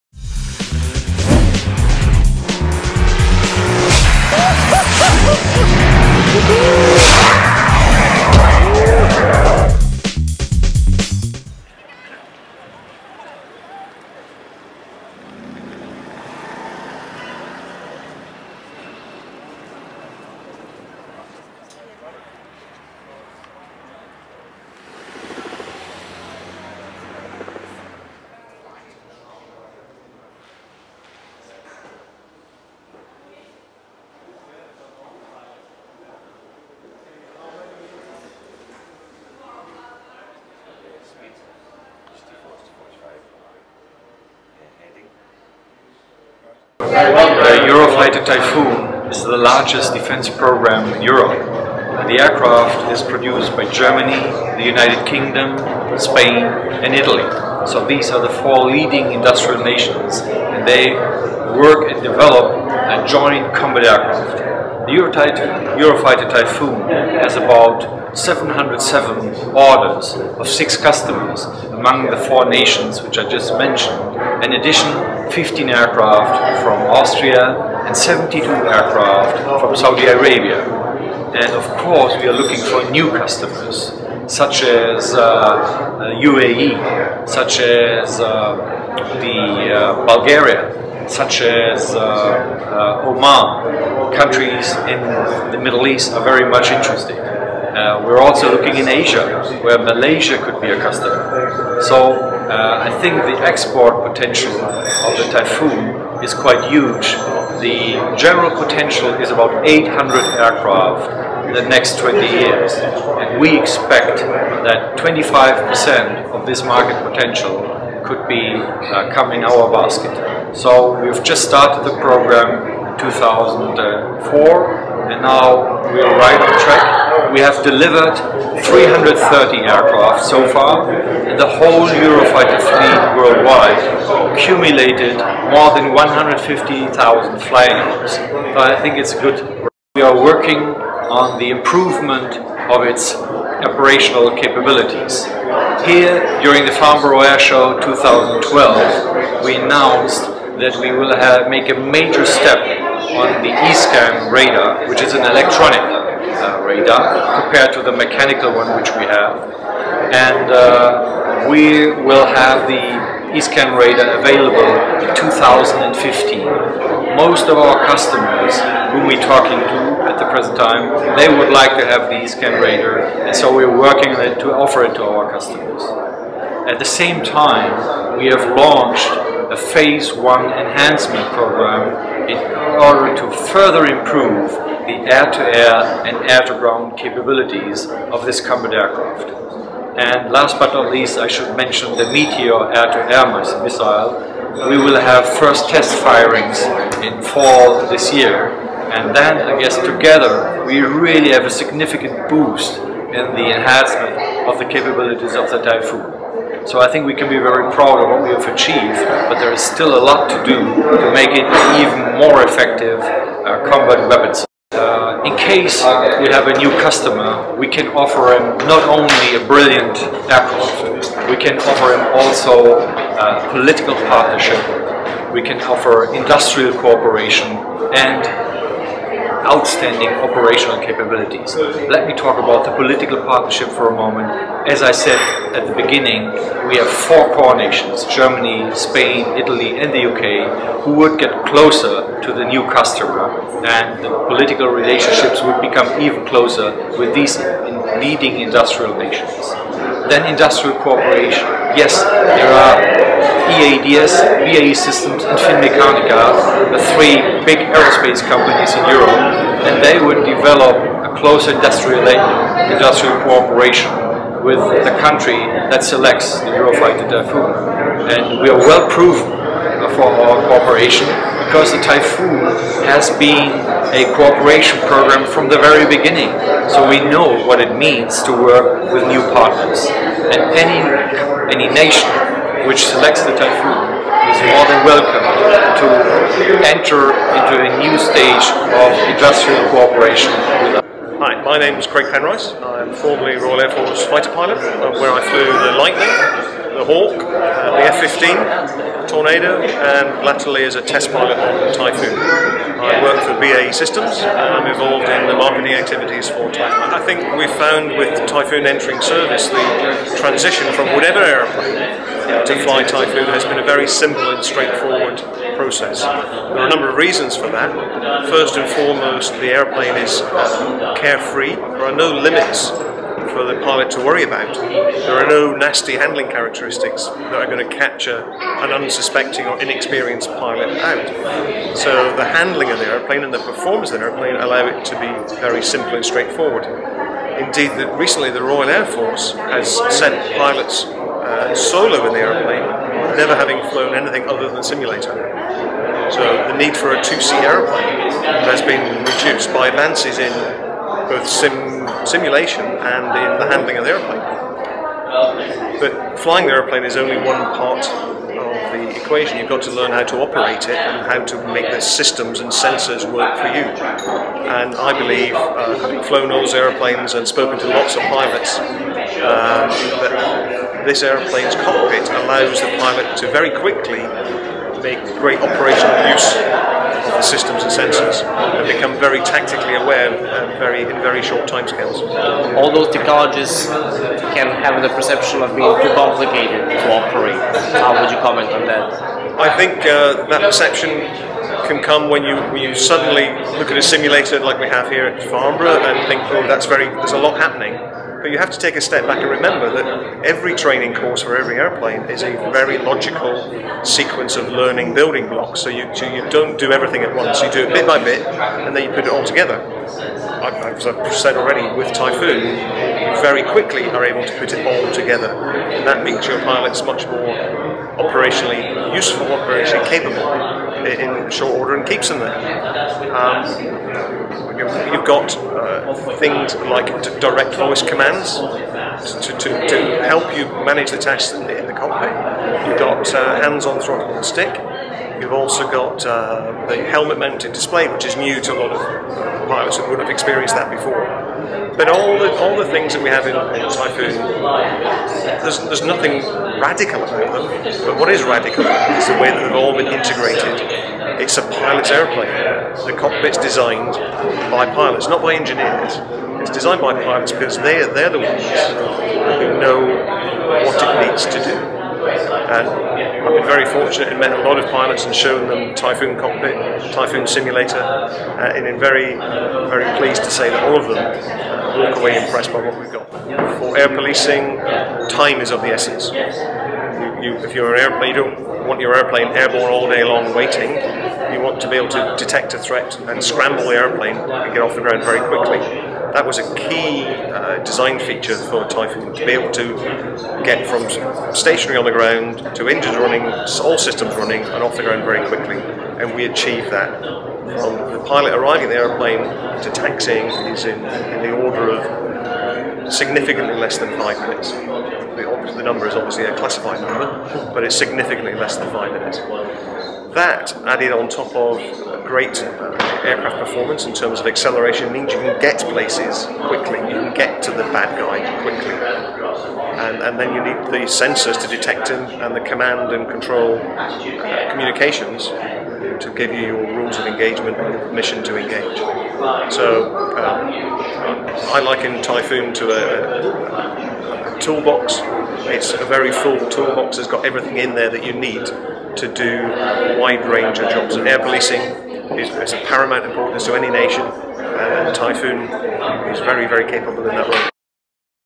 Eurofighter Typhoon (HD 720p)
Report on Eurofighter Typhoon presence at Farnborough International Airshow 2012. Interviews with Eurofighter's VP for PR & Communication and with the BAE Systems and Typhoon test pilot who was involved with the development of the aircraft from the beginning.